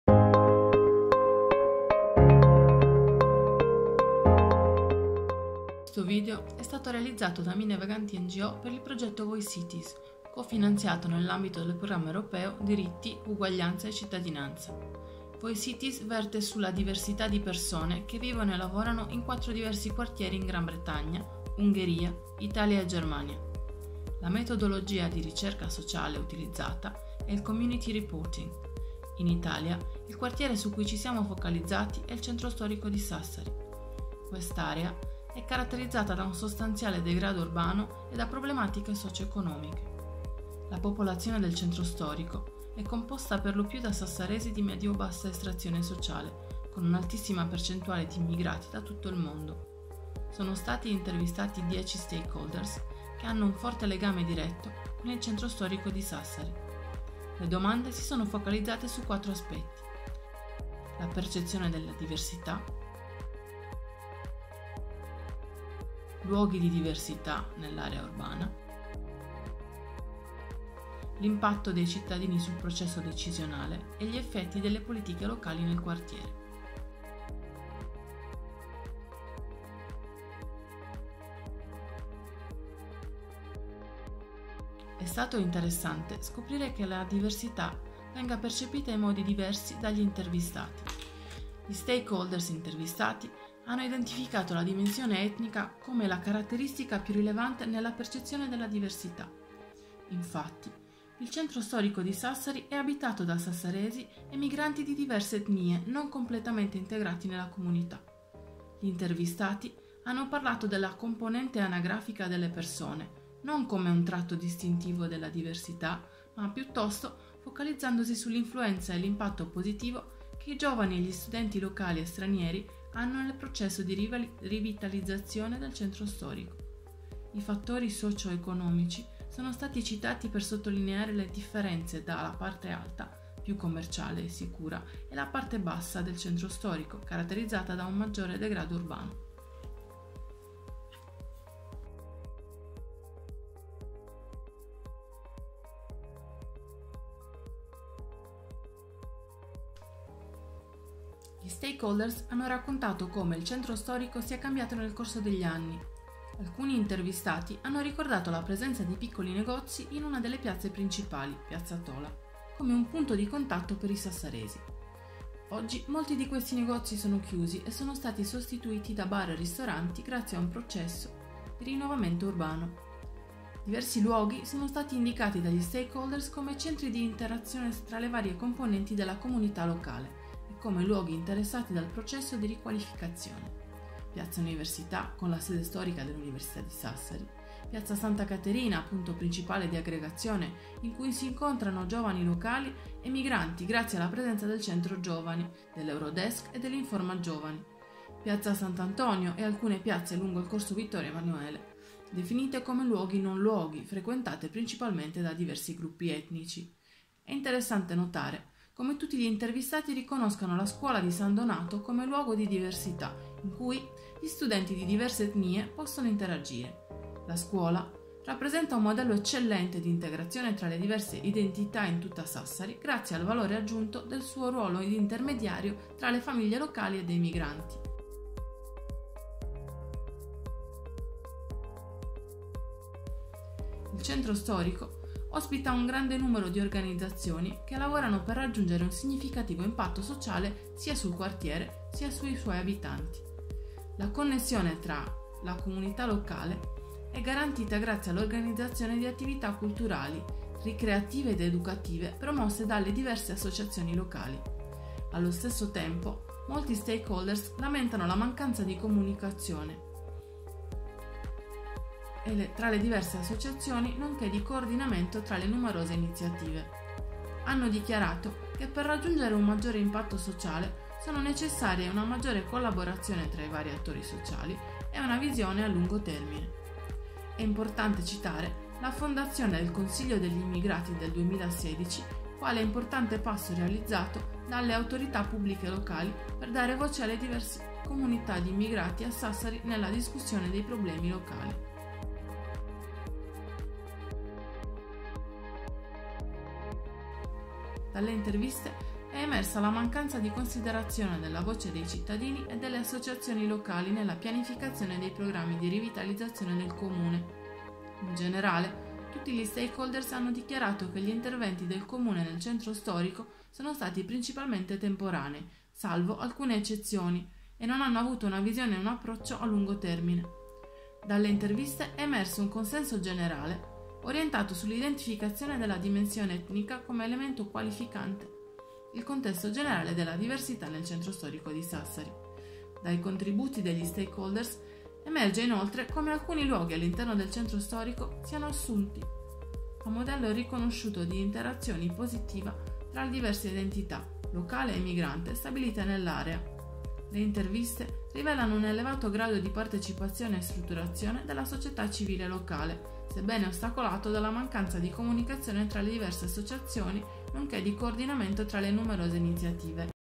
Voicitys- Local Stakeholders Interviews in Sassari - MVNGO
The video describes the process of interviews with local stakeholders, in the frame of the project Voicitys and in the context of Sassari. The video was made by Mine Vaganti NGO.